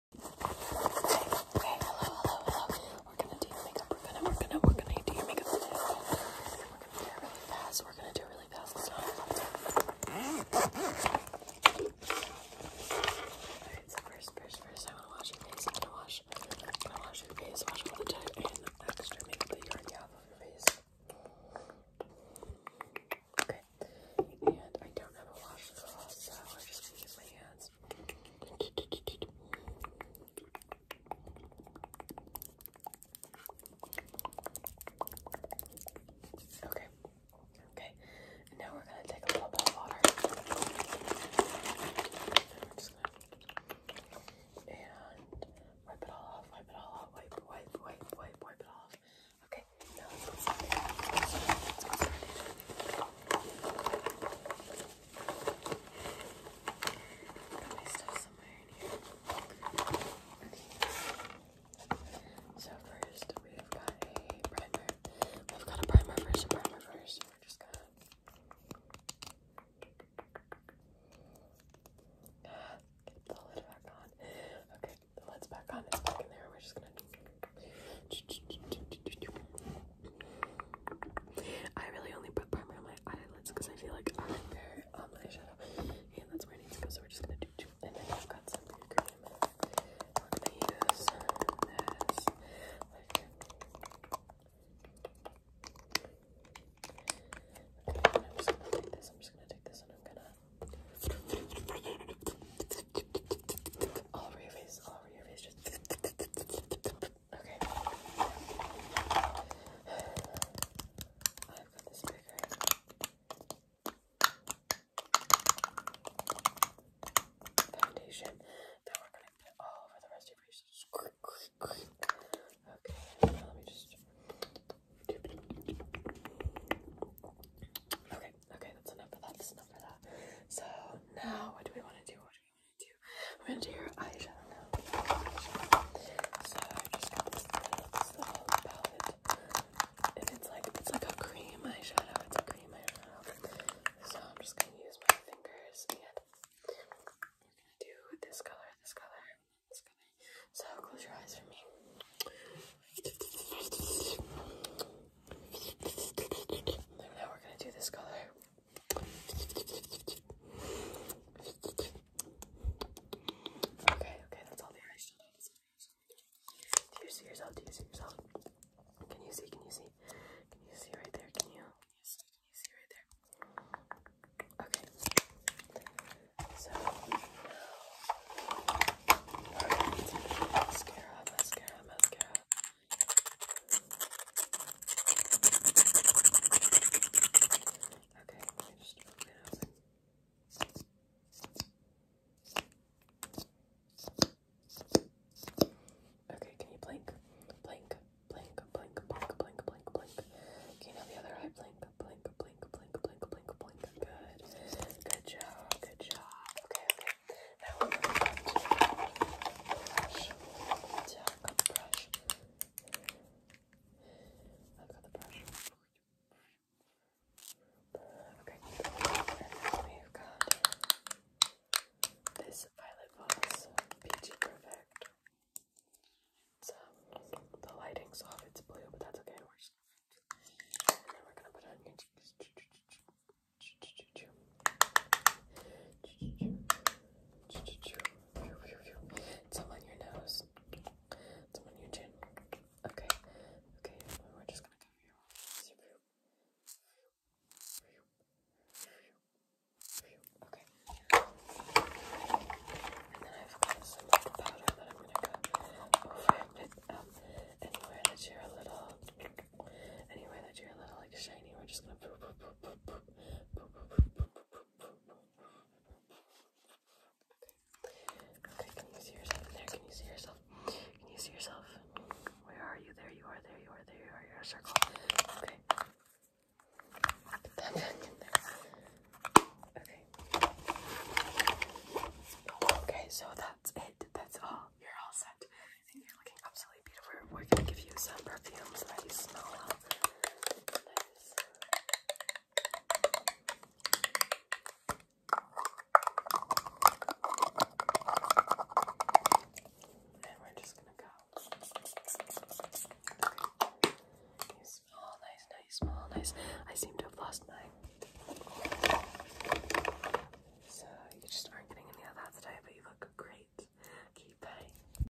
ASMR doing your makeup | fast and aggressive personal attention
Just some Lofi asmr for ya :)